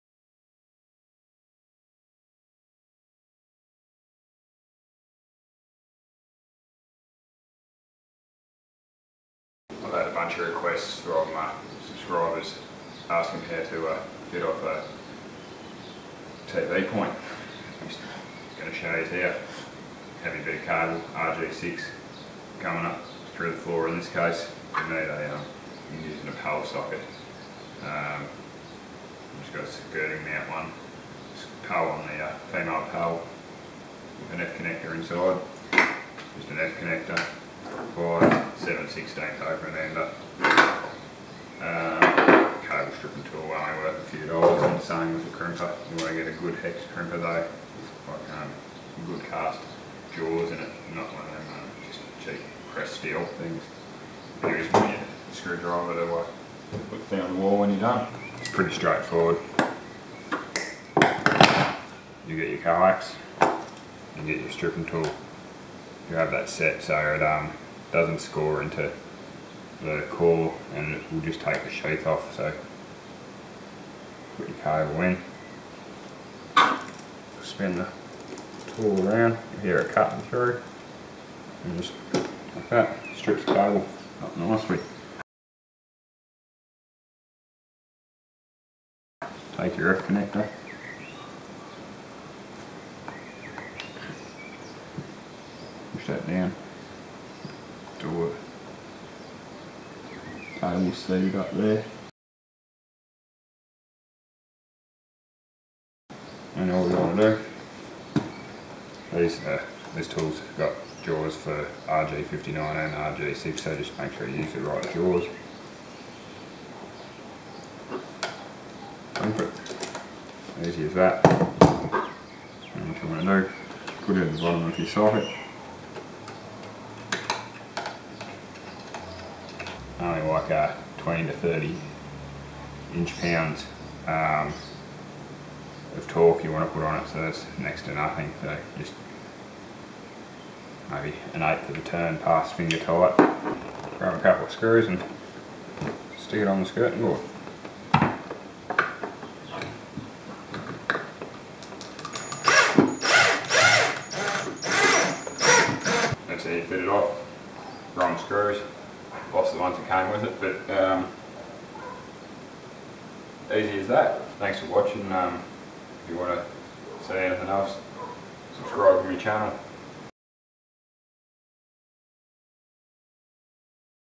How to install TV point using PAL / F Connector
A short video on how to install a TV connection using a PAL surface mount socket and F connector crimp termination. How to wire an F connector.

There is a get off point in the video after the crimping of the F Connector should you only be interested in that component.

All of the tools and materials can be found at your local electronics store or electrical wholesaler.